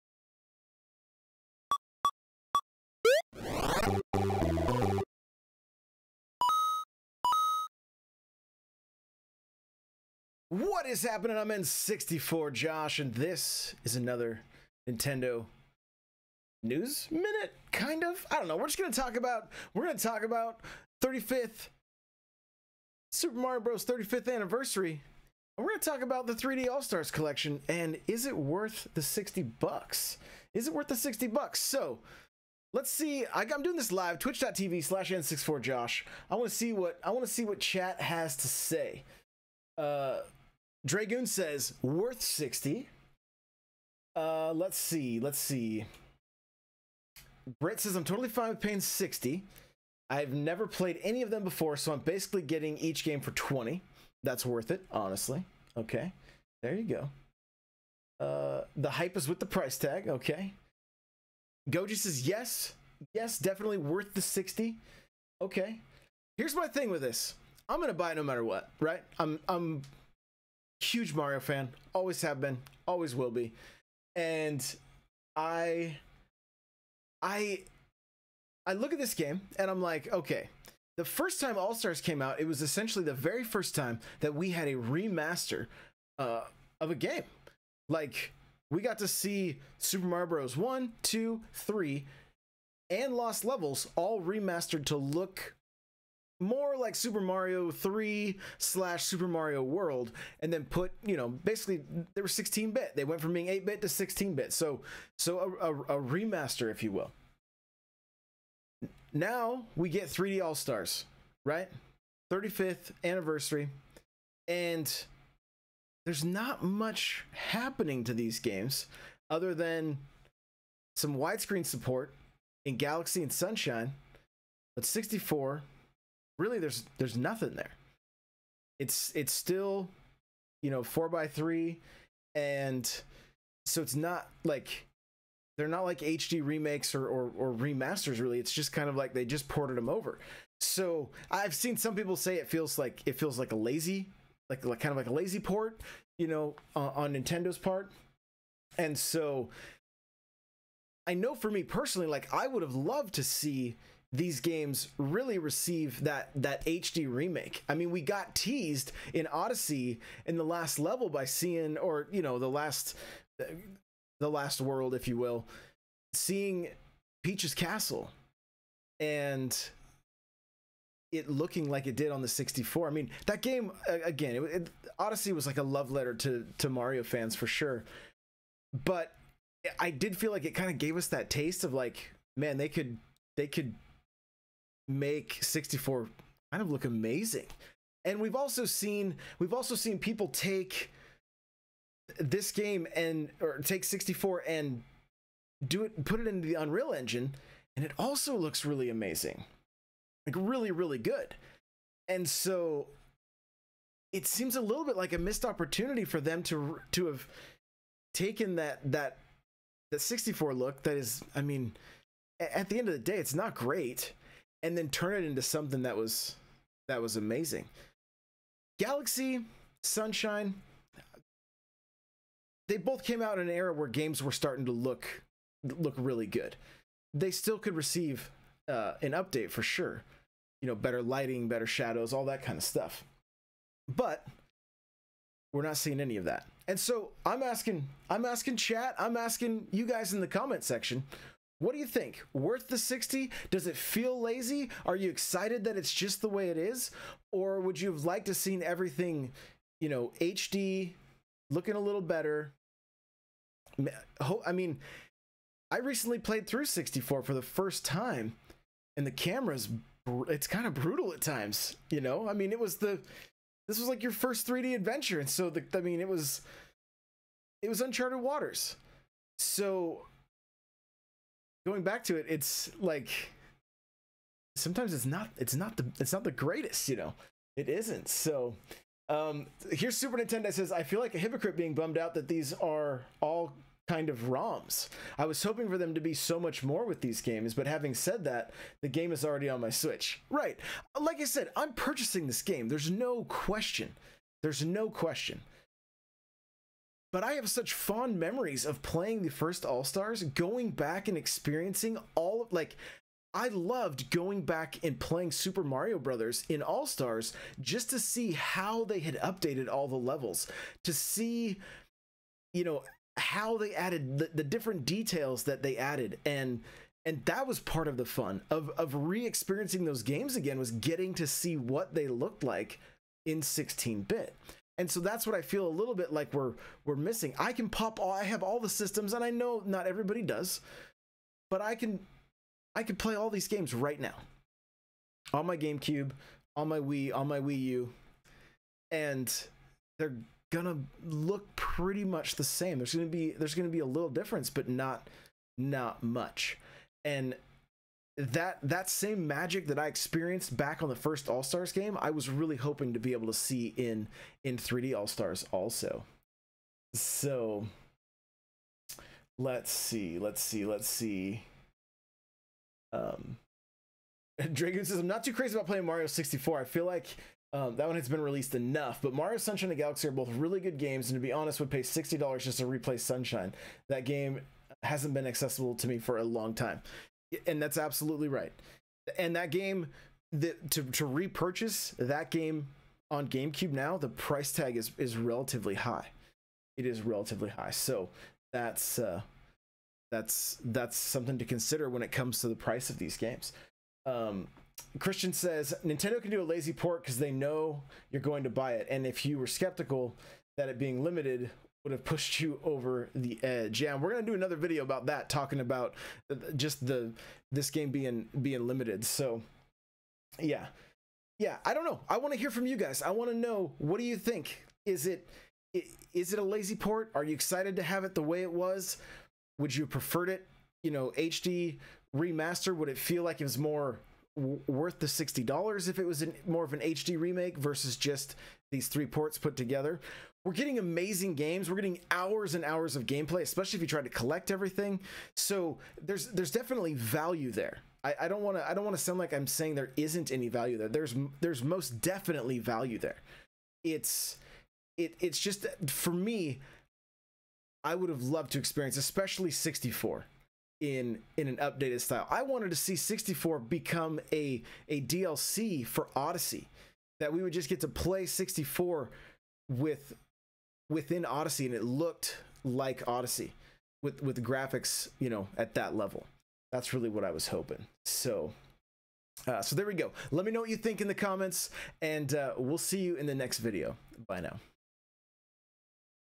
Super Mario 3D All-Stars: Is It Worth The $60?
It Super Mario 3D All-Stars a good value? Will you be picking it up on the 18th?